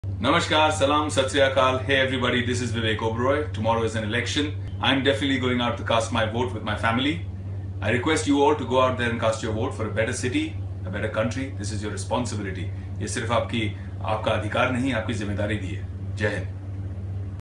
Know to WHOM Vivek Oberoi pass his Vote
In this short interview Mr. Vivek Oberoi gives us that information which information we wants to know, and Vivek Proudly pass the Information. We all follow the information as we love our country as like as Vivek Oberoi.